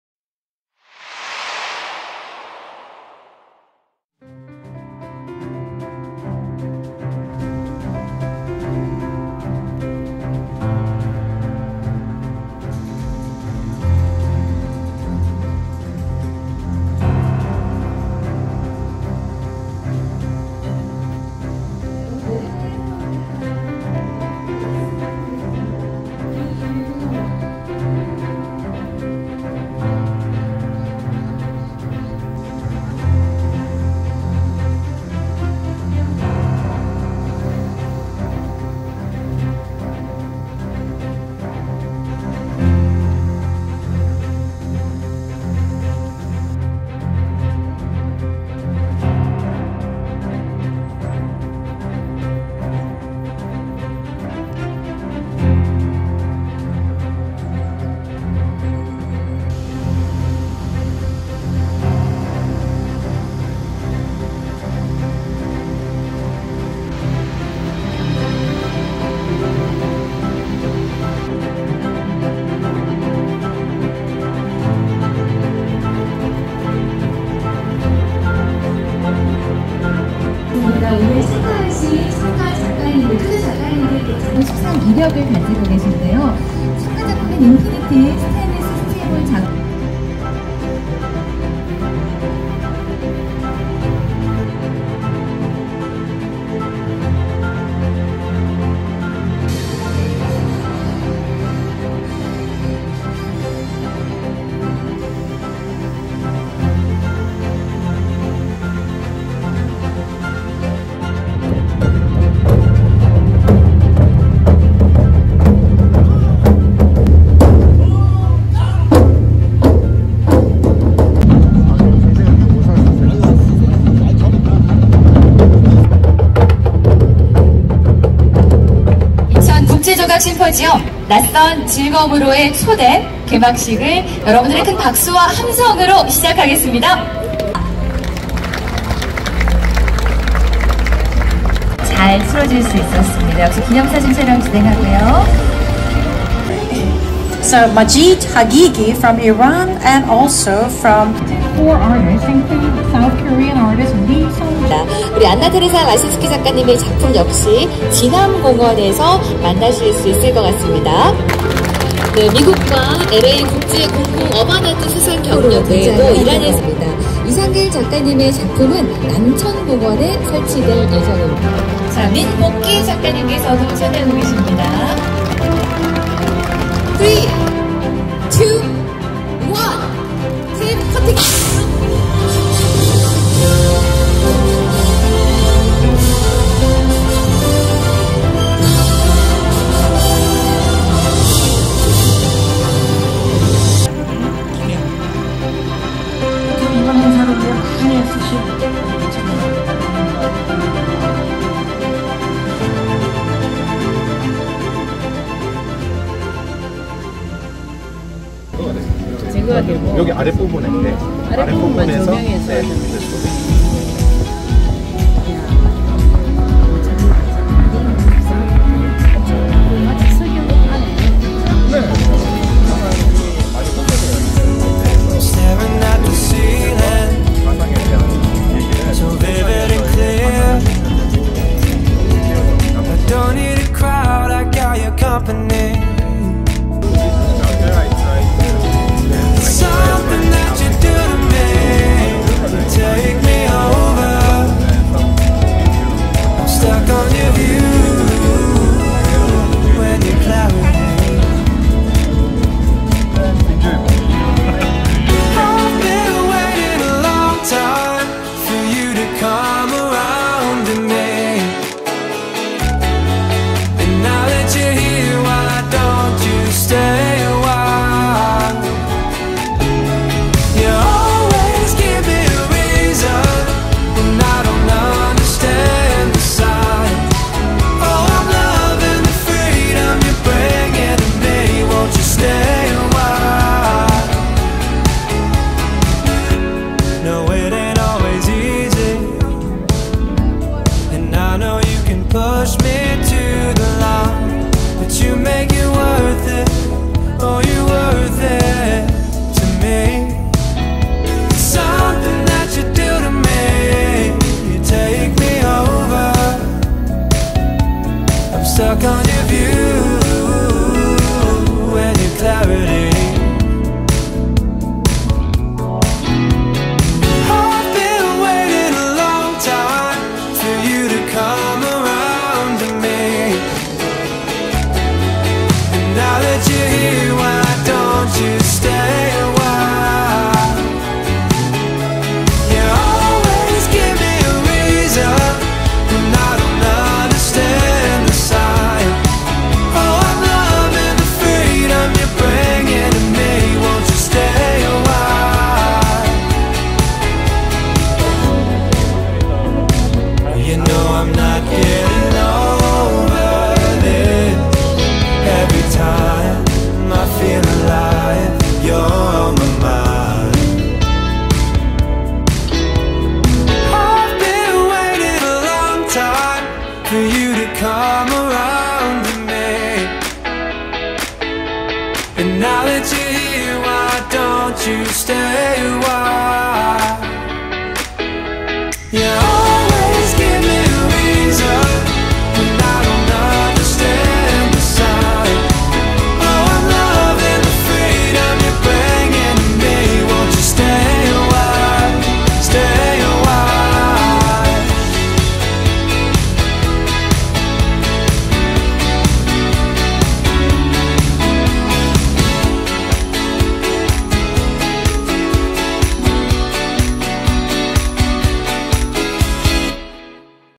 서울에서 당일치기로 가볼만한 곳 국내 저명한 작가들의 작품을 무료로 감상할 수 있는 이천국제조각심포지움
오늘은 서울에서 차로 1시간 거리에 당일치기로 가보기 좋은 특별한 전시가 열리고 있다고 해서 다녀왔습니다. 바로 올해로 26회를 맞는 이천국제조각심포지엄인데요. 국제적인으로 위상이 높은 행사입니다.

이번 이천국제조각심포지엄+33부스전은 이천에 위치한 서희테마파크에서 개최되고 있습니다.

이천국제조각심포지엄은 1998년 제1회 '사랑과 평화'라는 주제로 시작하여 지금까지 국내외 전문 조각가가 참여하는 이천의 대표적인 전시 조각 심포지엄입니다.
지난 25년간 매년 변화된 주제를 설정하여 공모를 통해 중견 및 신진작가 발굴에 기여하고 있습니다.

올해는 '낯선 즐거움으로의 초대' 라는 주제로 열려 이 중에 국내 두 분, 해외 두 분이 선정되었습니다.
4인의 작가로 선정된 분들 외에도 총 33개 부스에서 초대 작가의 작품들을 감상할 수 있는데,  개성있고 기발한, 또는 심오한 느낌의 작품들이 출품되었어요.

▶ 참여 작가 소개

1. 이송준 / 대한민국
금속 조각 전문 작가인 이송준은 '철'을 소재로 한 다양한 작품을 선보이는 작가입니다.
이번 전시에서 물리적인 공간에서의 무한한 반사를 통해 가상과 현실에 대한 이야기를 담을 예정입니다.

2. ANNA RASINSKA / 폴란드
주어진 시공간에서 스스로 이해하려 노력한다는 작가 안나 라신스카
그림자와 빛의 놀이를 통해 새로운 내용과 의미를 찾는  작품을 선보일 예정입니다.

3. Majid Haghighi  / 이란
기술과 인공지능, 전통 조각 기술의 시너지를 탐구하는 작가 마지드 하기기
인공지능 시스템이 인간과 기계 사이의 인지 과정을 매끄럽게 융합되는 크리스탈 넥서스를 선보입니다.

4. 이상길 / 대한민국
2000년 대한민국 미술대전 대상과 미국 프리먼재단의 '아시아 작가상'을 수상한 거장입니다.
이번 전시에는 자연 현상이 만들어 낸 조약돌의 이미지를 형상화한 작품을 선보이는데,  시간의 흐름과 인고의 의연함을 작품에 녹여낸다고 합니다.

이번 행사는 일반적인 전시와는 다르게 조각 작품의 생성 과정을 눈앞에서 생생하게 볼 수 있으며
이곳에서 탄생한 작품들은 이천의 명소들에 설치될 예정입니다.

제 26회 이천국제조각심포지엄+33부스전은 2024년 8월 19일부터 9월 9일까지 22일간 개최되며
8월 23일엔 개막식이, 9월 9일에는 폐막식이 열립니다.

▶ 33부스 전시

국내외에서 열심히 작품 활동을 하고 있는
작가님들의 작품을 볼 수 있는 전시 부스입니다.

지금까지 일상이여행이 다녀온 이천국제조각심포지엄이었습니다.
좀처럼 보기 힘든 작품들을 9월 9일까지 관람이 가능하오니 이곳에 오셔서 문화와 감성을 충전하시기 바랍니다.

▶ 행사 정보
- 장소 : 서희역사관 (경기 이천시 부발읍 무촌로 18번길 130)
- 행사 일정 8월 19일 ~ 9월 9일
  개막식 8월23일 17:00 / 폐막식 9월9일 17:00
- 매주 금,토 오전 10시 30분 도슨트프로그램 운영
- 8월 31일(토) 16:00 조각교양강좌
- 9월 3일 (화) 09:30 작가와 함께하는 문화탐방 (선착순15명)

위 영상은 이천국제조각심포지엄으로부터 제작지원을 받아 제작되었습니다.